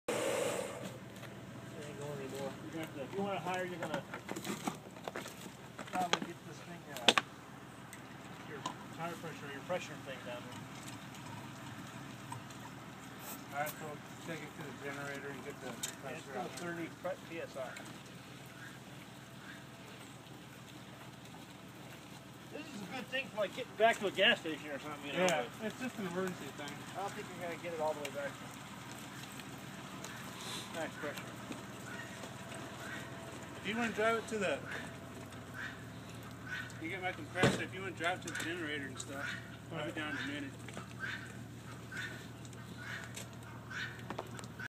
Life at the watering hole
Just like in the wild - a lot goes on at the watering hole on Shofar Mountain.
I wished I had started filming two minutes earlier - you would have seen Sister Jaimie walking away with a 5 gallon bucket of water in each hand (yes, they were full).